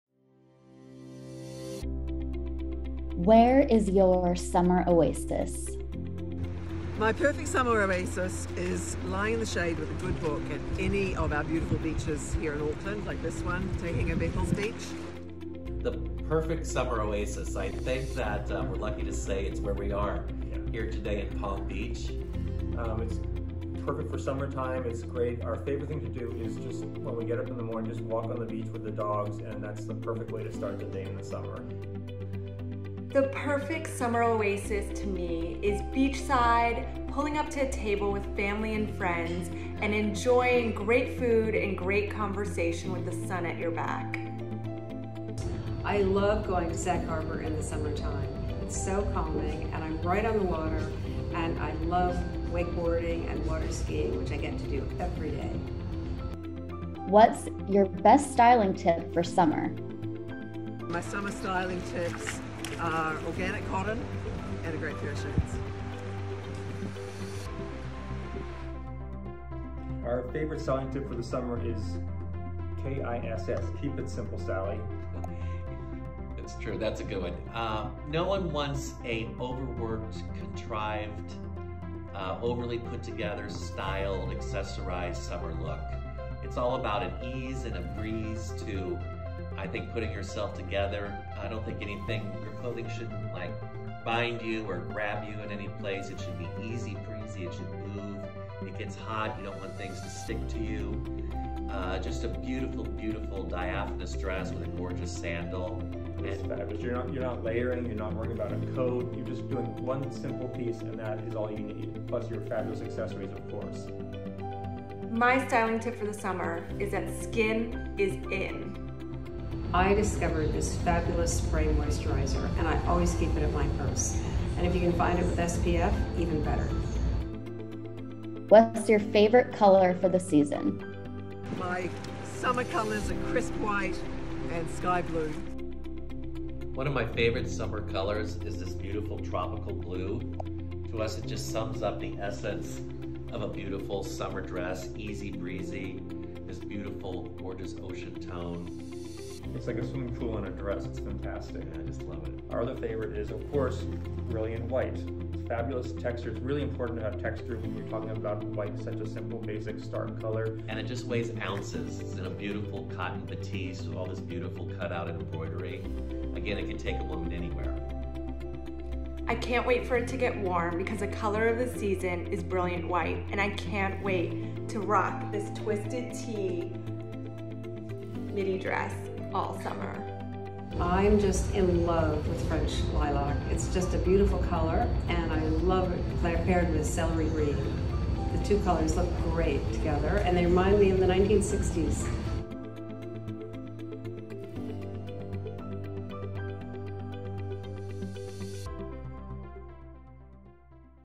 Summer Styling Tips With Covet Brands
This summer, we’re encouraging Coveters to discover your oasis with Covet Fashion. We spoke with some of our favorite brands to get the best summer styling tips for in-game and IRL! Check out the video and then get styling for summer!